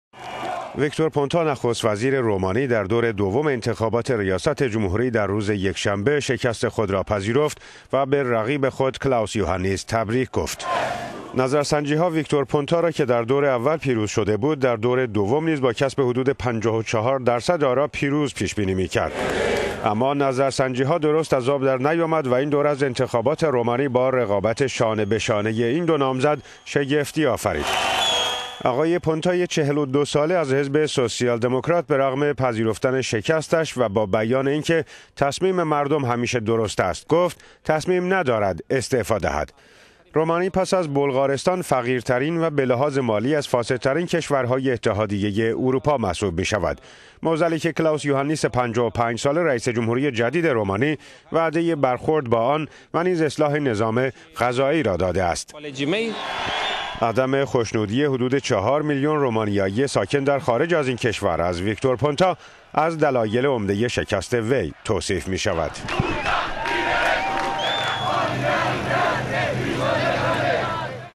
رومانی؛ پونتا بر خلاف نظرسنجی ها از یوهانیس شکست خورد
ویکتور پونتا، نخست وزیر رومانی در دور دوم انتخابات ریاست جمهوری در روز یکشنبه شکست خود را پذیرفت و به رقیب پیروز خود کلاؤس یوهانیس تبریک گفت.

نظرسنجی ها، ویکتور پونتا را که در دور اول پیروز شده بود در دور دوم نیز با کسب حدود ۵۴ درصد آراء پیروز پیش بینی می کرد.

اما نظرسنجی ها درست از آب در نیامد و این دور از انتخابات رومانی با رقابت شانه به شانه این دو نامزد شگفتی آفرید.

آقای پونتای ۴۲ ساله از حزب سوسیال دموکرات رومانی برغم پذیرفتن شکست خود و با بیان این که «تصمیم مردم همیشه درست است» گفت قصد …